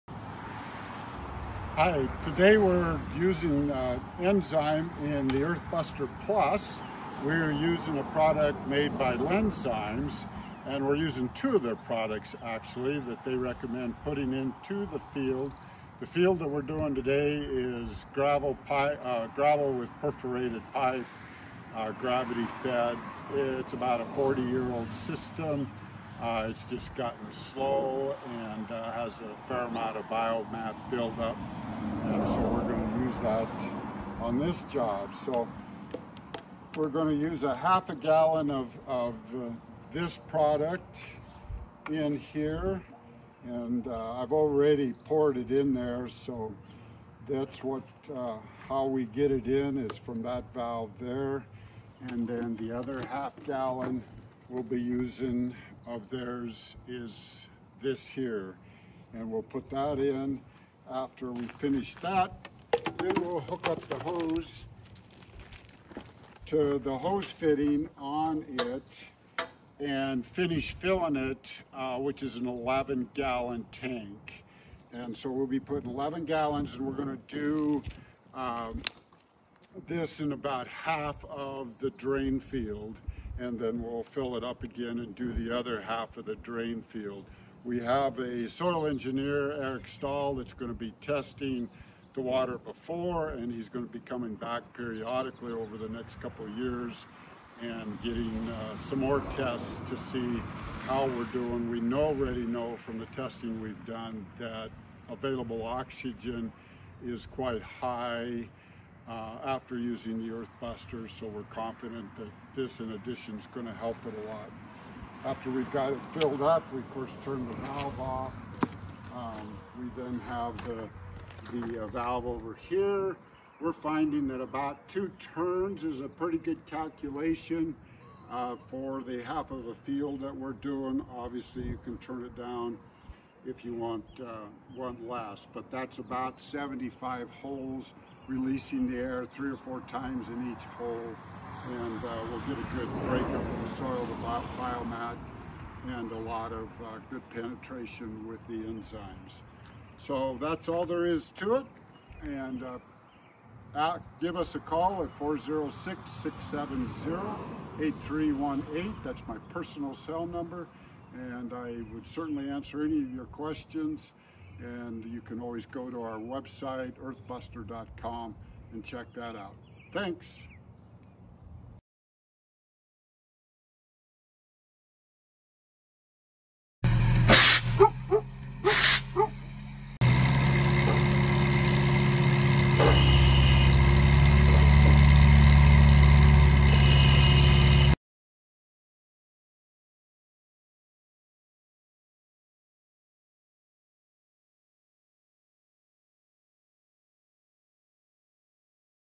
EarthBusterPLUS is easy to use.
Today we are using Lenzyme additives in and septic drain field and test results.